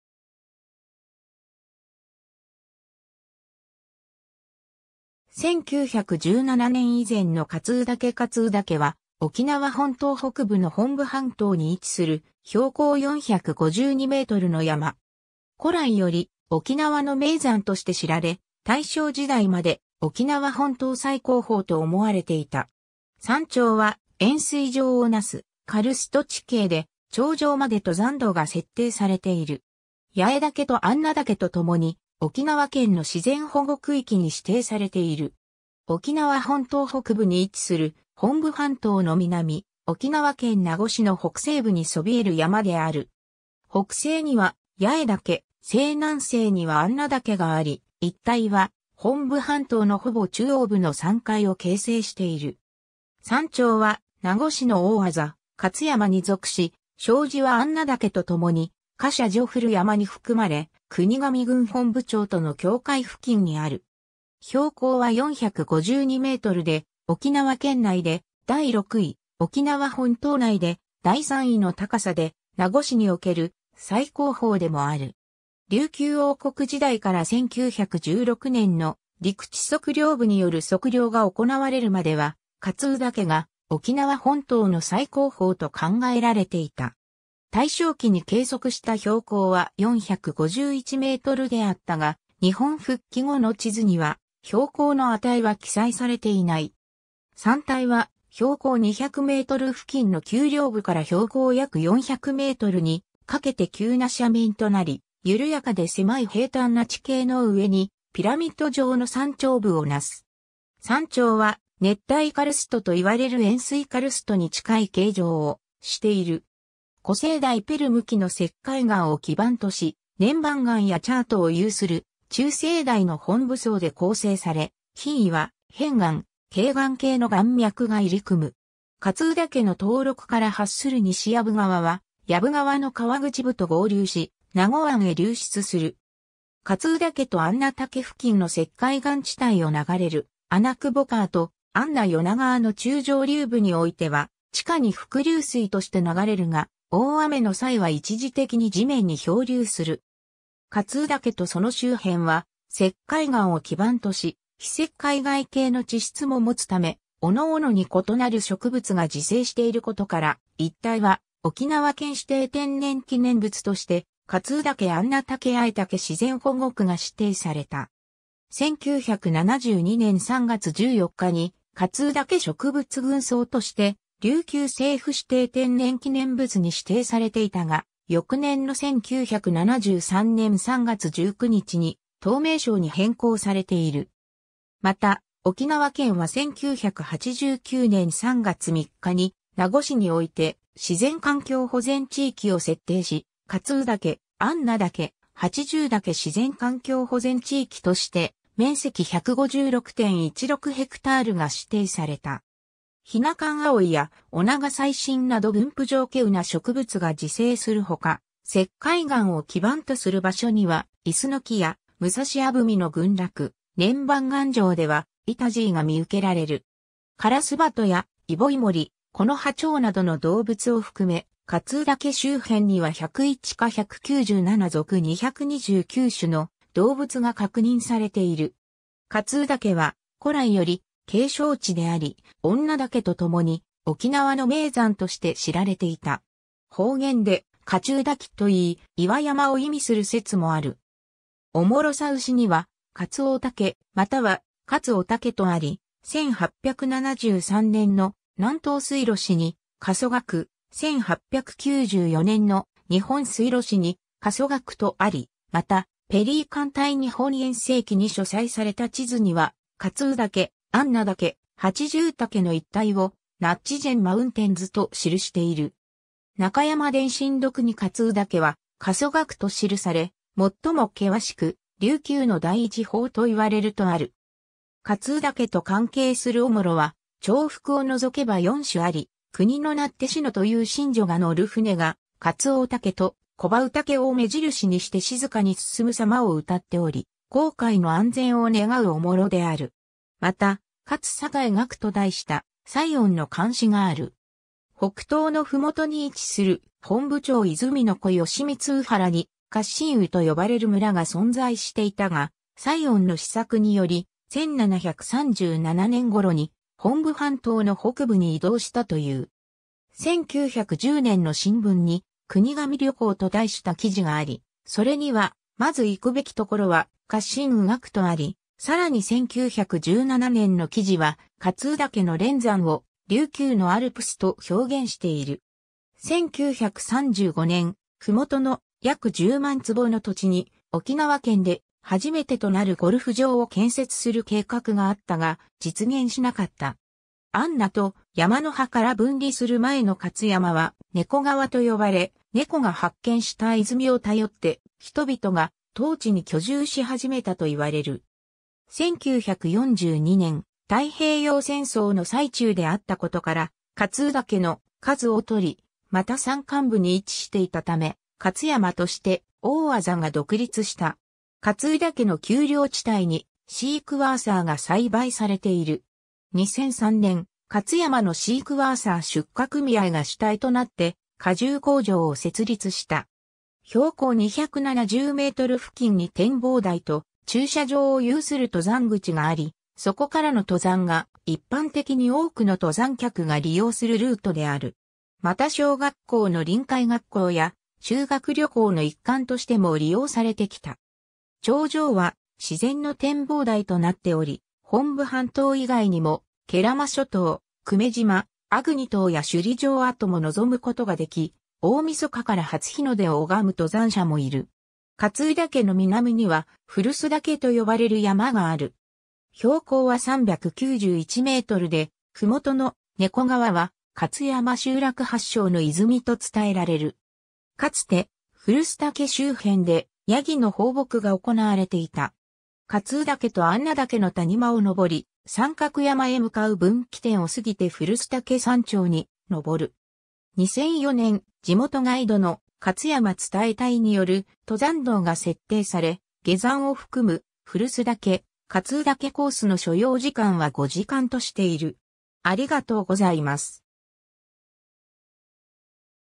嘉津宇岳
1917年以前の嘉津宇岳 嘉津宇岳（かつうだけ）は、沖縄本島北部の本部半島に位置する、標高452メートルの山。
 古来より沖縄の名山として知られ、大正時代まで沖縄本島最高峰と思われていた。
山頂は円錐状をなすカルスト地形で、頂上まで登山道が設定されている。
八重岳と安和岳とともに沖縄県の自然保護区域に指定されている。
 沖縄本島北部に位置する本部半島の南、沖縄県名護市の北西部にそびえる山である。
北西には八重岳、西南西には安和岳があり、一帯は本部半島のほぼ中央部の山塊を形成している。
山頂は名護市の大字「勝山（かつやま）」に属し、小字は安和岳と共に「我謝如古山（ガジャナクヤマ）」に含まれ、国頭郡本部町との境界付近にある。
 標高は452メートルで、沖縄県内で第6位、沖縄本島内で第3位の高さで、名護市における最高峰でもある。
琉球王国時代から1916年（大正5年）の陸地測量部による測量が行われるまでは、嘉津宇岳が沖縄本島の最高峰と考えられていた。
大正期に計測した標高は451メートルであったが、日本復帰後の地図には標高の値は記載されていない。
 山体は標高200メートル付近の丘陵部から標高約400メートルにかけて急な斜面となり、緩やかで狭い平坦な地形の上にピラミッド状の山頂部をなす。
山頂は熱帯カルストといわれる円錐カルストに近い形状をしている。
古生代ペルム紀の石灰岩を基盤とし、粘板岩やチャートを有する中生代の本部層で構成され、ひん岩・片岩・珪岩系の岩脈が入り組む。
 嘉津宇岳の東麓から発する西屋部（にしやぶ）川は、屋部川の河口部と合流し、名護湾へ流出する。
嘉津宇岳と安和岳付近の石灰岩地帯を流れる穴窪川と安和与那川の中上流部においては、地下に伏流水として流れるが、大雨の際は一時的に地面に表流する。
 嘉津宇岳とその周辺は石灰岩を基盤とし、非石灰岩系の地質ももつため、各々に異なる植物が自生していることから、一帯は沖縄県指定天然記念物（天然保護区域）として、「嘉津宇岳安和岳八重岳自然保護区」が指定された。
1972年（昭和47年）3月14日に「嘉津宇岳植物群叢」として琉球政府指定天然記念物に指定されていたが、翌年の1973年（昭和48年）3月19日に当名称に変更されている。
また、沖縄県は1989年（平成元年）3月3日に名護市において自然環境保全地域を設定し、「嘉津宇岳・安和岳・八重岳自然環境保全地域」として面積156.16ヘクタールが指定された。
 ヒナカンアオイやオナガサイシンなど分布上稀有な植物が自生するほか、石灰岩を基盤とする場所にはイスノキやムサシアブミの群落、粘板岩上ではイタジイが見受けられる。
カラスバトやイボイモリ、コノハチョウなどの動物を含め、嘉津宇岳周辺には101科197属229種の動物が確認されている。
 嘉津宇岳は古来より景勝地であり、恩納岳と共に沖縄の名山として知られていた。
 方言で「カチュウダキ」といい、「岩山」を意味する説もある。
『おもろさうし』には「かつおうたけ」、または「かつおたけ」とあり、1873年（明治6年）の『南島水路誌』に「佳蘇（カソ）嶽」、1894年（明治27年）の『日本水路誌』に「佳楚（カソ）嶽」とあり、また『ペリー艦隊日本遠征記』に所載された地図には、嘉津宇岳・安和岳・八重岳の一帯を「NATCHIJIN MOUNTAINS （ナチジン山地）」と記している。
『中山伝信録』に嘉津宇岳は「佳楚嶽」と記され、「最も険しく、琉球の第一峰といわれる」とある。
 嘉津宇岳と関係するおもろは重複を除けば4首あり、「くにのなてしの」という神女が乗る船が、「かつおうたけ（嘉津宇嶽）」と「こばうたけ（蒲葵嶽）」を目印にして静かに進むさまを謡っており、航海の安全を願うおもろである。
また、「勝宇嶽」と題した蔡温の漢詩がある。
 北東の麓に位置する、本部町伊豆見の古嘉津宇原に、「嘉津宇」と呼ばれる村が存在していたが、蔡温の施策により、1737年（乾隆2年）頃に本部半島の北部に移動したという。
1910年（明治43年）の新聞に「国頭旅行」と題した記事があり、それには「まづ行くべき所は嘉津宇嶽」とあり、さらに1917年（大正6年）の記事は、嘉津宇岳の連山を「琉球のアルプス」と表現している。
1935年（昭和10年）、麓の約10万坪の土地に、沖縄県で初めてとなるゴルフ場を建設する計画があったが、実現しなかった。
「安和」と「山入端」から分離する前の勝山は、「猫川（マヤーガー）」と呼ばれ、猫が発見した泉を頼って...